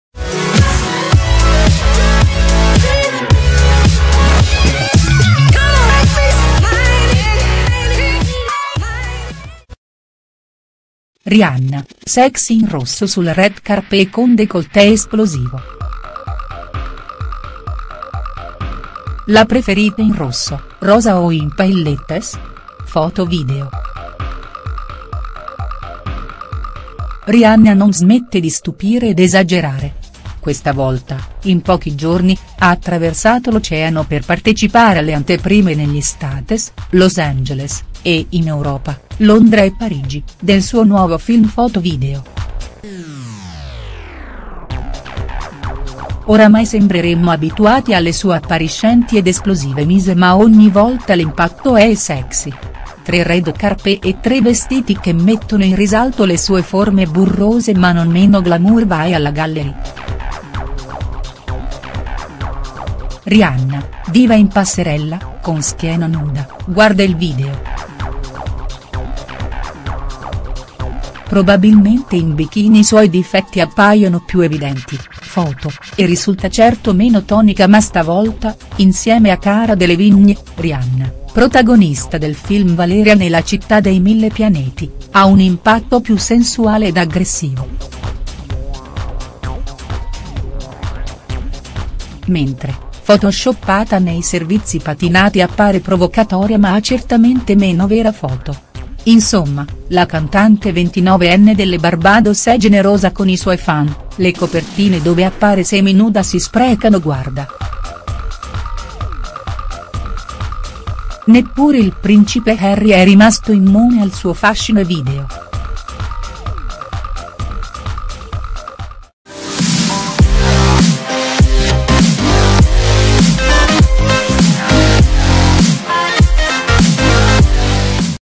Rihanna, sexy in rosso sul red carpet con décolleté esplosivo.Full HD
La bella Rihanna una e trina: in pochi giorni, con look diversi ma sempre provocanti, passa sui red carpet di mezzo mondo. Il motivo? il lancio del suo ultimo films